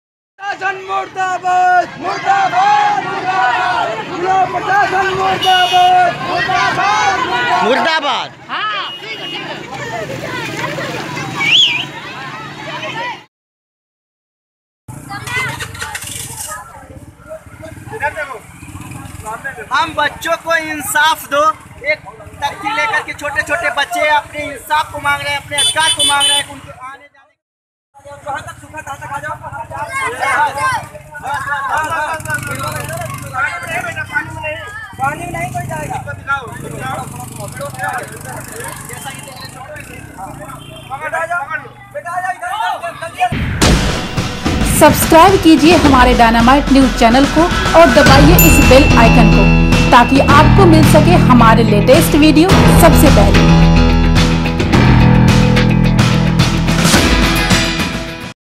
आजमगढ़: गड्ढे में तब्दील हुए केंद्रीय विद्यालय के रास्ते, फूट पड़ा बच्चों व अभिभावकों का गुस्सा
आजमगढ़: गड्ढे में तब्दील हुए केंद्रीय विद्यालय के रास्ते, फूट पड़ा बच्चों व अभिभावकों का गुस्सा, पौधे लगाकर जताया विरोध
Full Story: